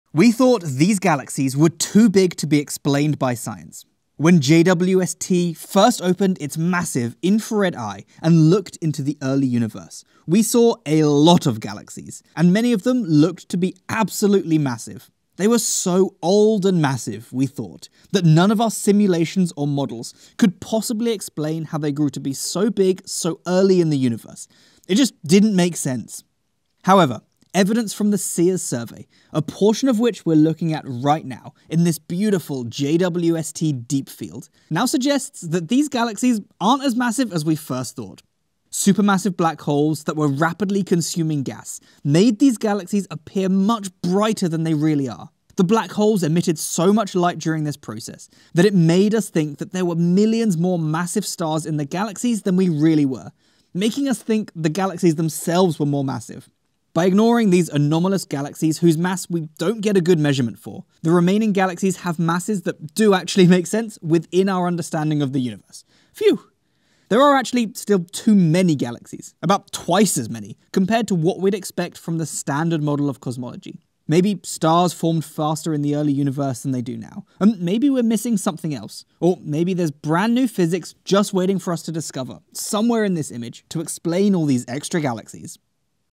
JWST Didn't Break Cosmology!
JWST did not break cosmology! We thought galaxies spotted by JWST we too massive to be explained, but supermassive black holes explain it all! How? Watch to find out!

CEERS Image Credit: NASA, ESA, CSA, Steve Finkelstein (UT Austin)

Hey team!